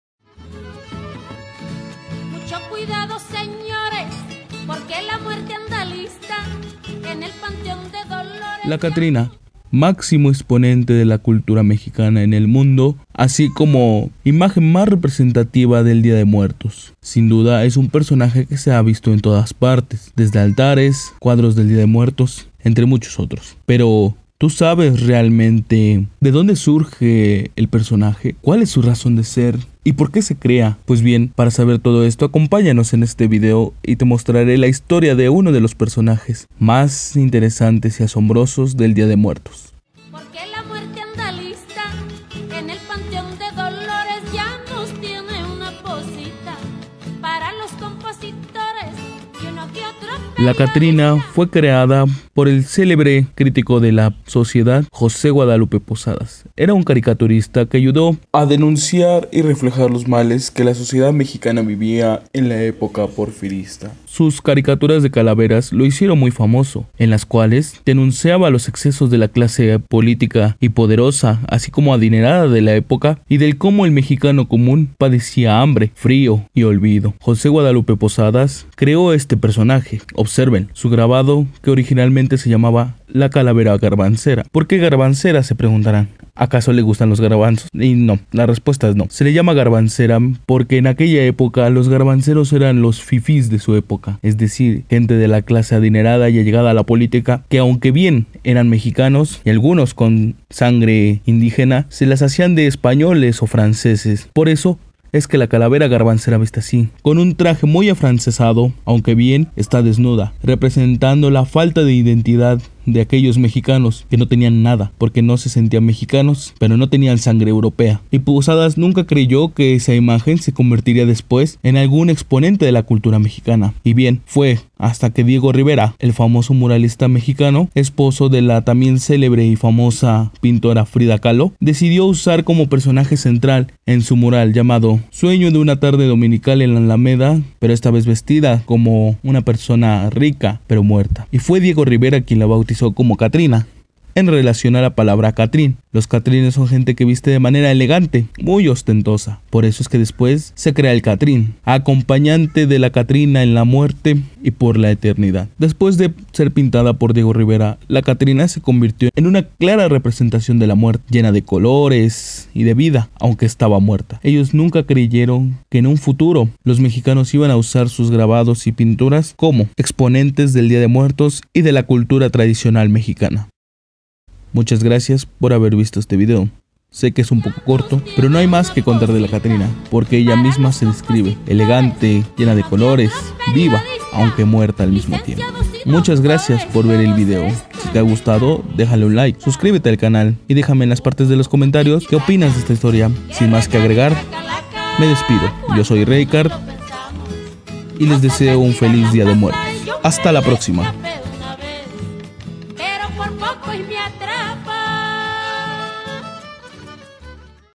Historia de la Catrina (Calavera Garbancera)
En este video te narro un poco de la historia de creación de uno de los simbolos más representativos del día de muertos "La Catrina"...  recuerda que tambien nos puedes encontrar y seguir en nuestras redes sociales: